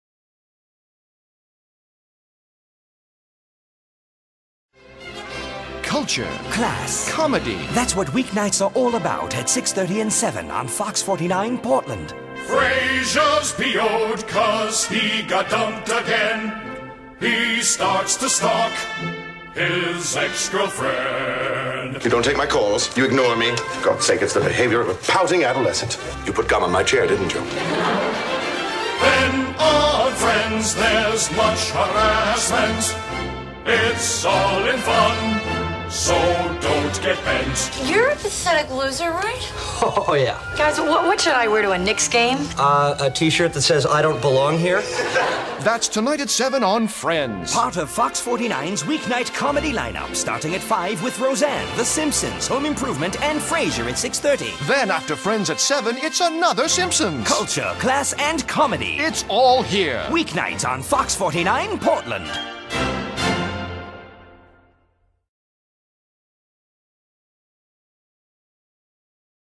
Russ Lindway radio campaign: Culture, Class & Comedy 'B'.wmv
Radio spot campaign for KPDX-TV
Writer/Producer/Voice: Russ Lindway
Voice: Chris Mezzolesta
Sound Designer: Jeff Gates/Commercial Sound + Image